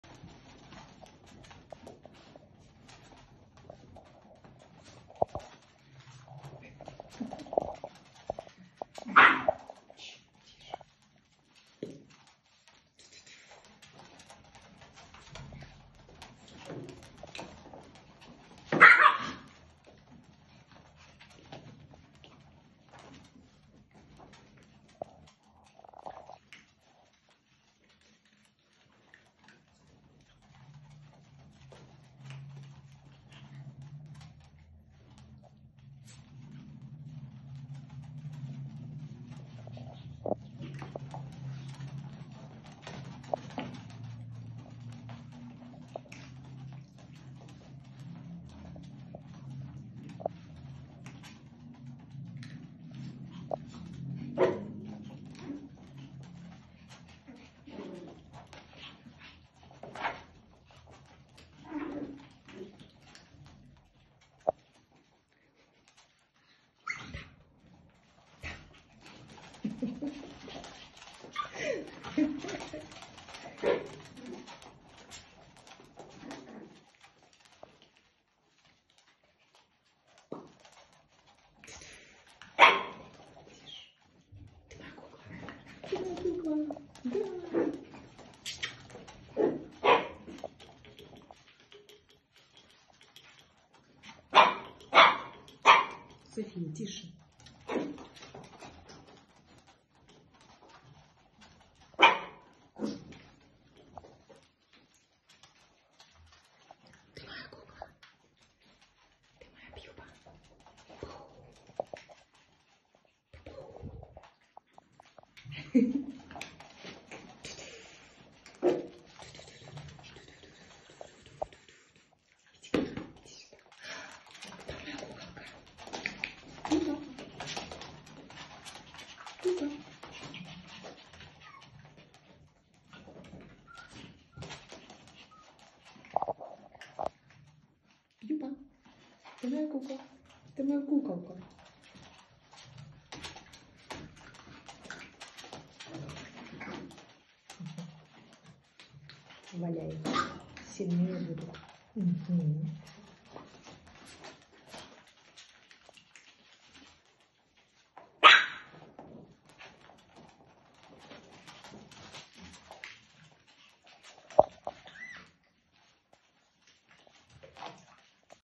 Питомник щенков мальтезе Аэлит.
Щенки мальтийской болонки мини в питомнике в Москве. Приезжайте, выбирайте своего самого-самого волшебного мальтези ))